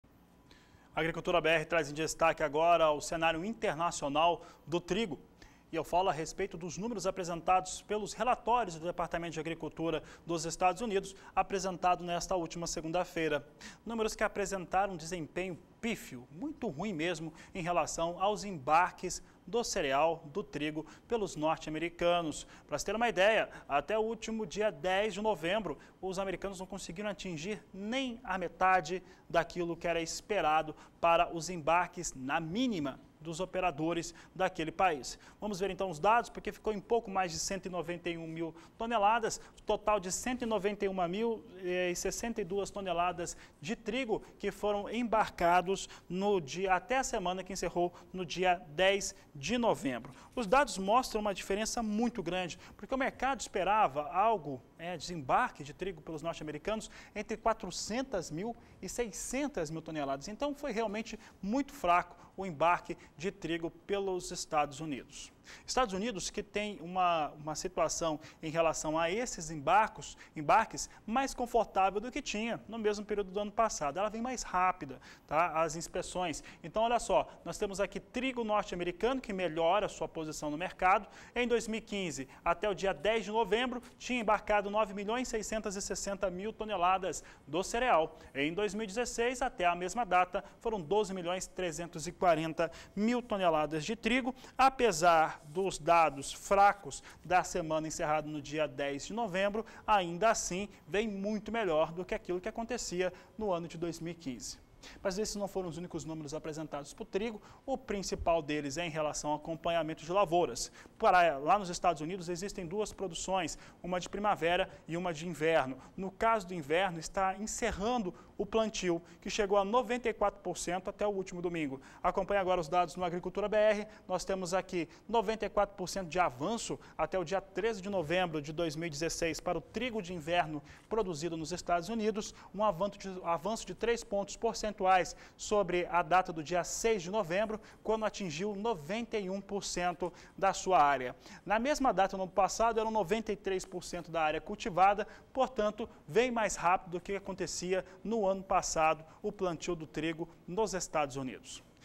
TRIGO - 15/11/2016
Embarques de trigo dos EUA ficam 60% abaixo da mínima esperada pelo mercado

Ainda de acordo com números do USDA para embarques, os números do trigo vieram muito fracos e a última semana apenas 191,062 mil toneladas foram inspecionadas. O volume ficou bem abaixo do esperado, já que o mercado apostava em algo entre 400 mil e 600 mil toneladas. Os EUA já embarcaram, em todo o ano comercial 2016/17, 12.347,367 milhões de toneladas, acima das 9.666,847 milhões do mesmo período do 2015/16.

USDA informa que 94% das áreas de trigo de inverno dos EUA foram semeadas

O departamento de agricultura dos EUA divulgou, no final da tarde de segunda-feira, os números do relatório de acompanhamento de lavouras. Para o trigo de inverno, trouxe avanço de 3 p.p., atingindo 94% da área a ser plantada. A média dos últimos cinco anos é de 95%, enquanto em 2015, na mesma data, os norte-americanos já haviam atingido 93% do plantio de trigo.